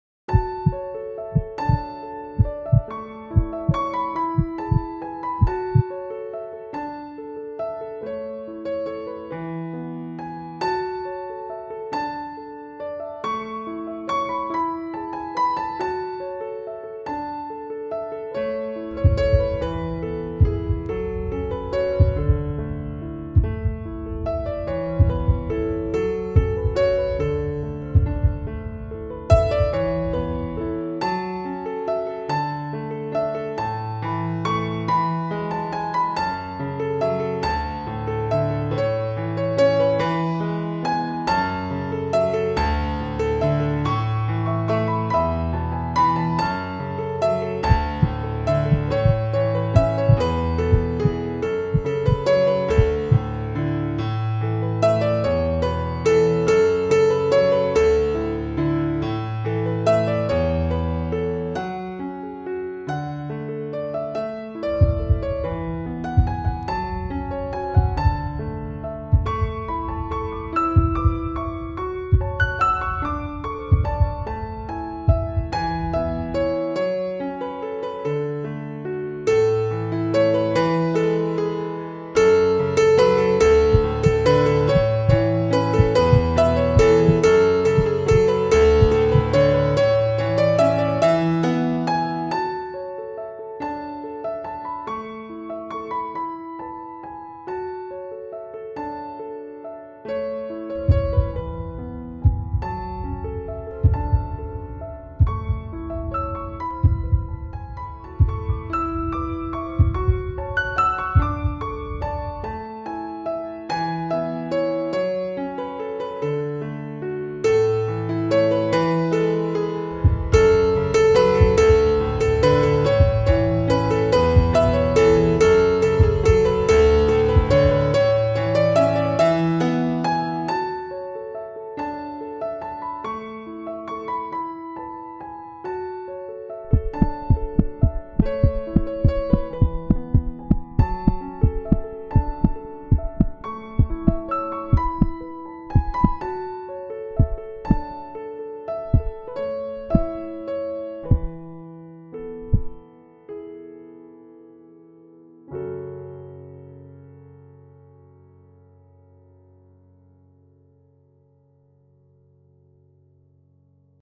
INTRO Corazones latiendo ELEMENT 3D | AFTER EFFECTS
INTRO de corazones que palpitan. La escena esta creada con Cinema 4D, se trata de una escena de un segundo, en el próximo tutorial os dejaré la escena para que puedas utilizarla libremente. la intro esta realizada con Element 3D desde el que importamos la escena de Cinema 4D y con After Effects realizamos toda la postproducción.

Espero les Guste este Vídeo, Déjame un Like !